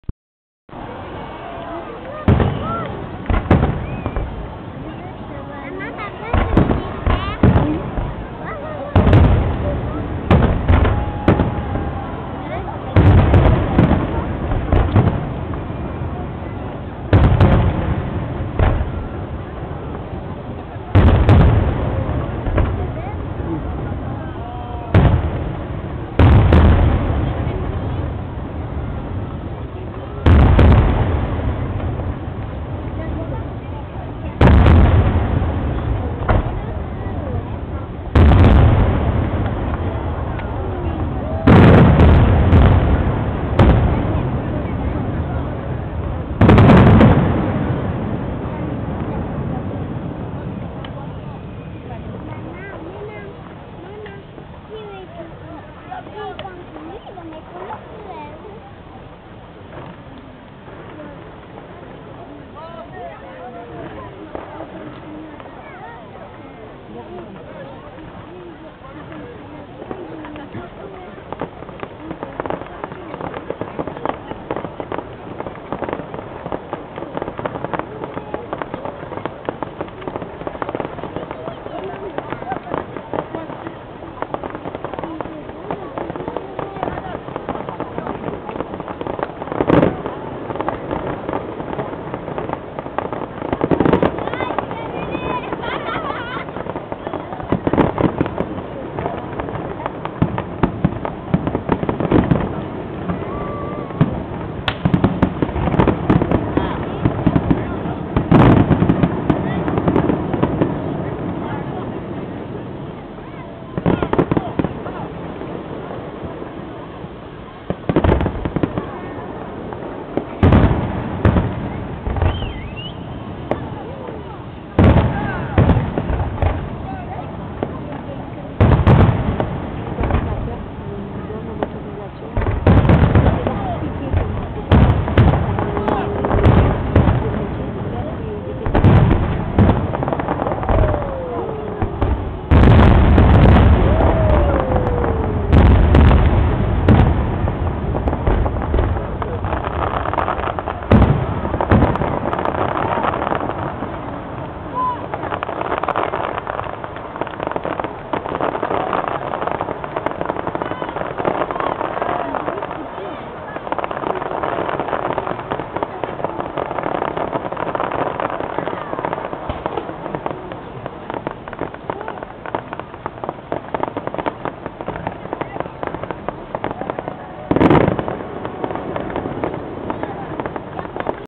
Fireworks feux d'artifice du 14 Juillet 2007 à Marseille 01
Le feu d'artifice de Marseille en France pour la fête nationale du 14 Juillet 2007 - Fireworks from Marseille France July 14 2007